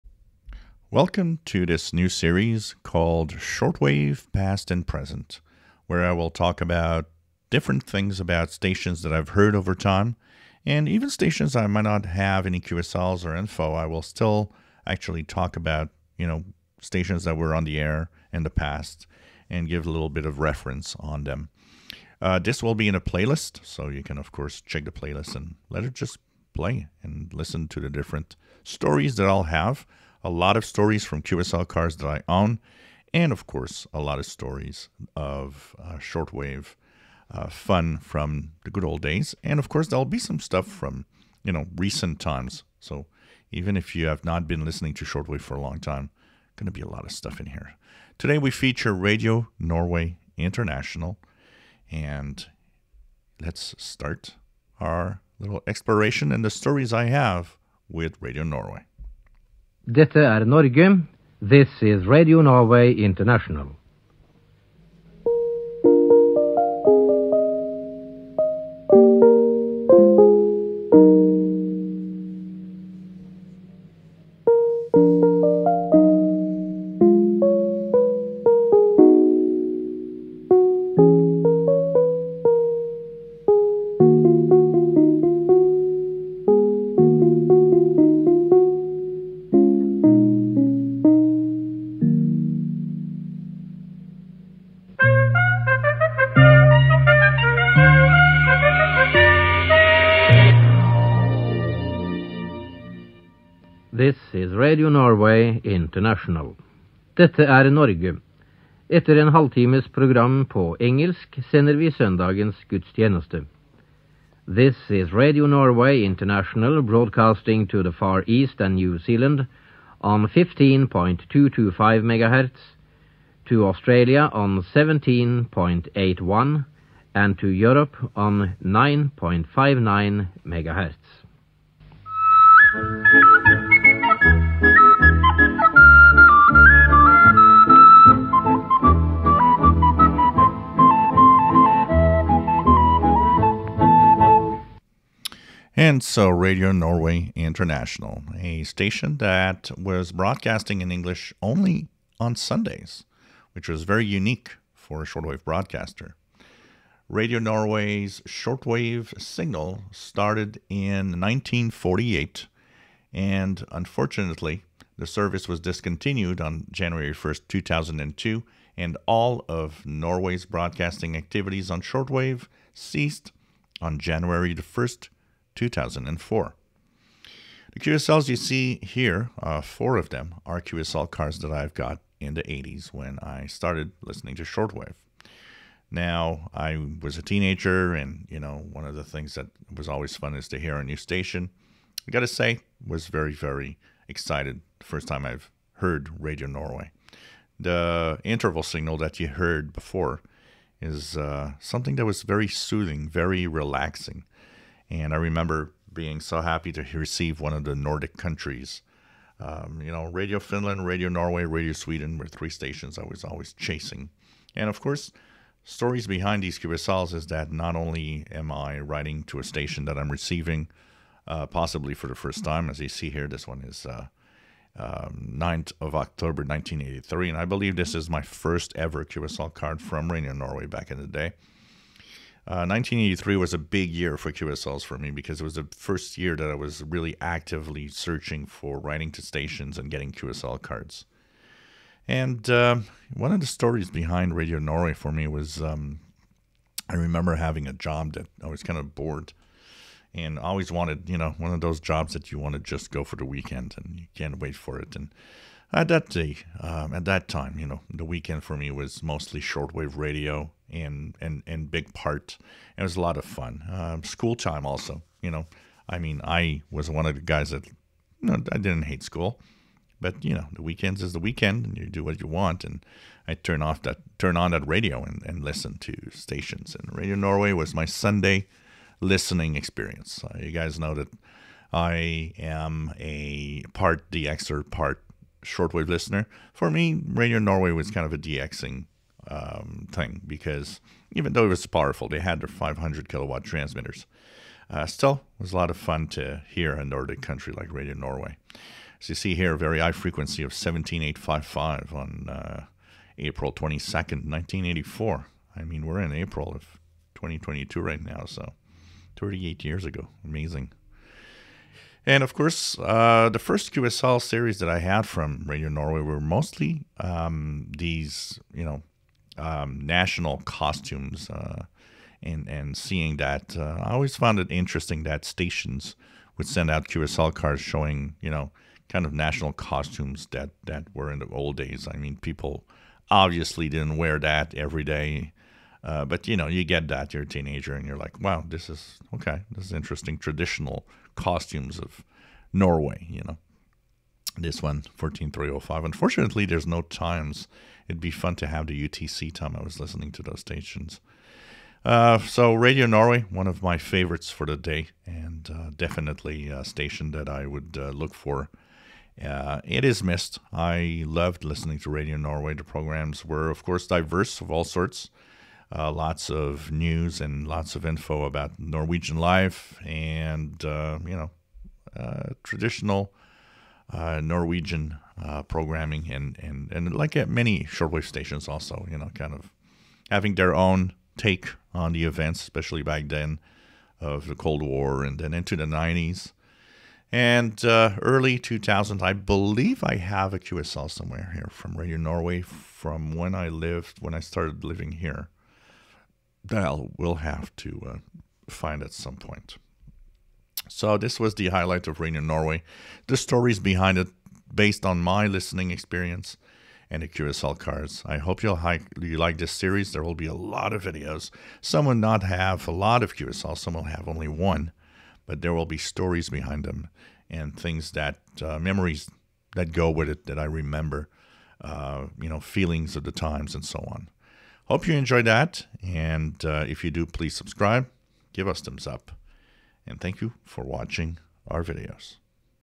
Radio Norway International Shortwave past and present series
Interval signal, brief history and personal observations and stories of Radio Norway International